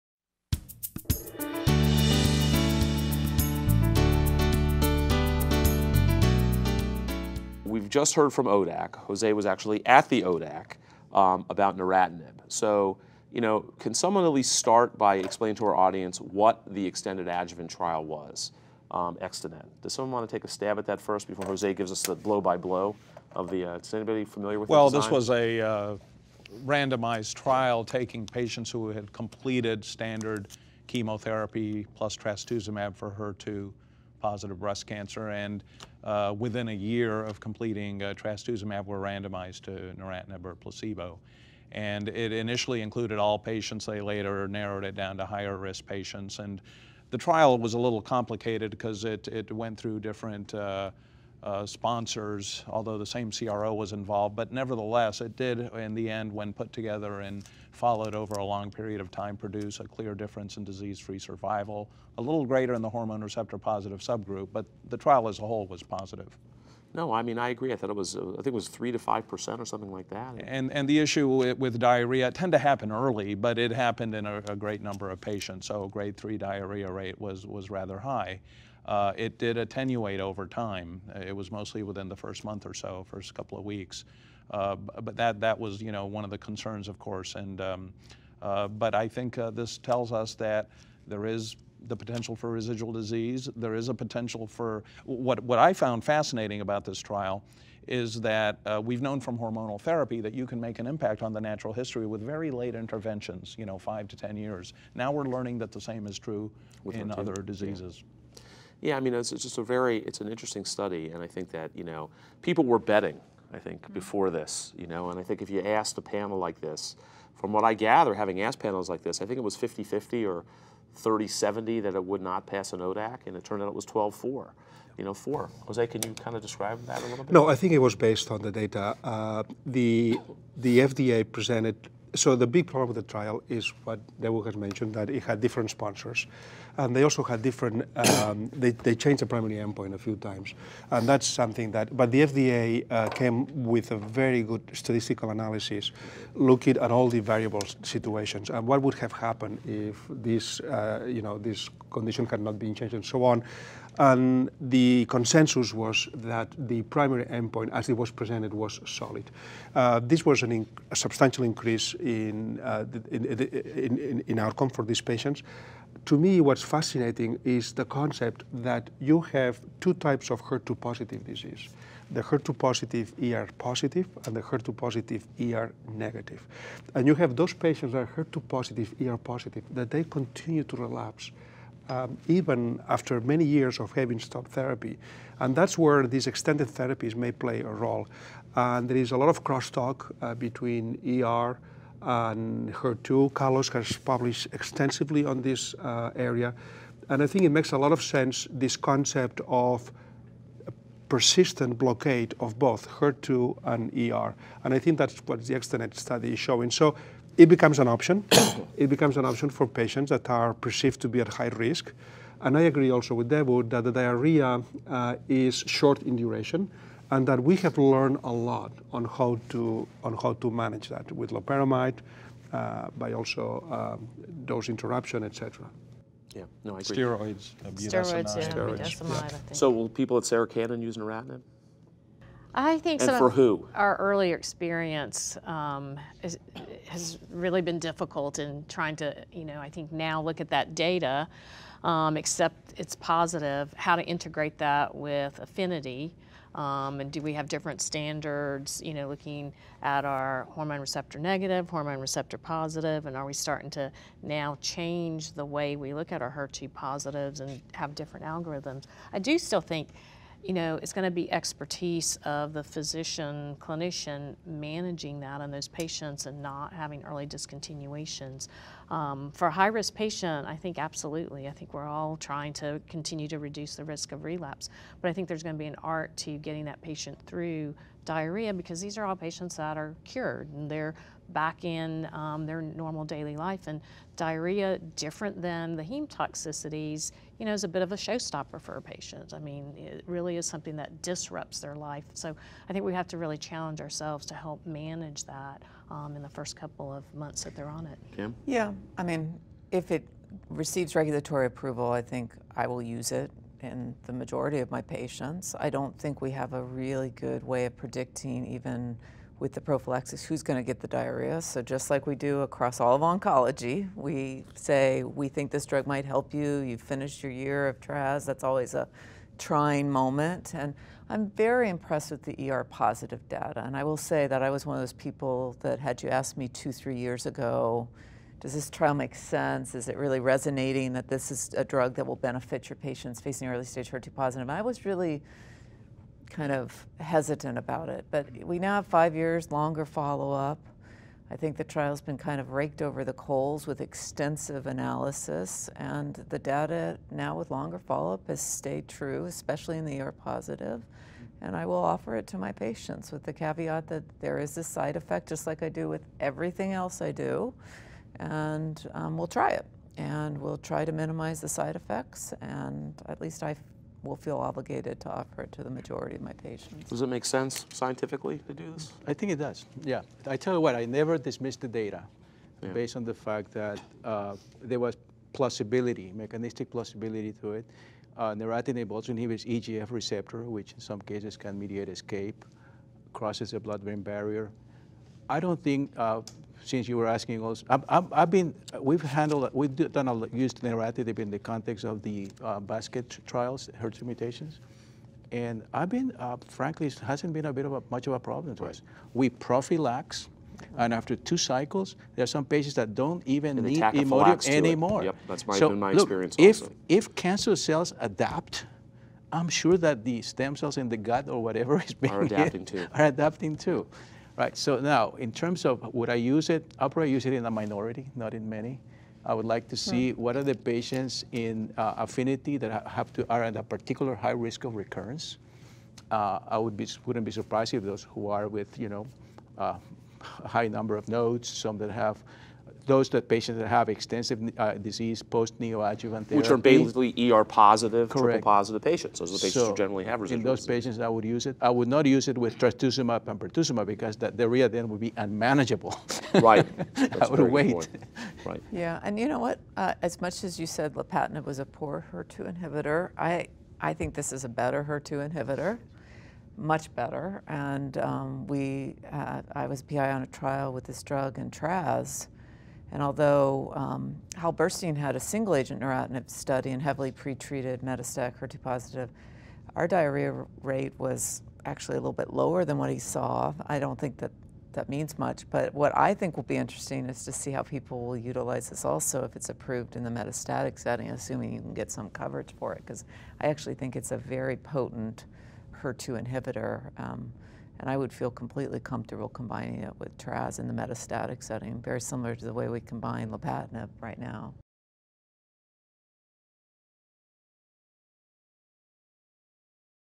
Extended Adjuvant Therapy for HER2-Positive Breast Cancer
Panelists Adam M. Brufsky, MD, PhD; Debu Tripathy, MD; José Baselga, MD, PhD; Denise A. Yardley, MD; Kimberly L. Blackwell, MD; and Carlos L. Arteaga, MD, discuss using neratinib for extended adjuvant therapy in patients with HER2-positive early-stage breast cancer and the recommendations for FDA approval from the recent ODAC meeting.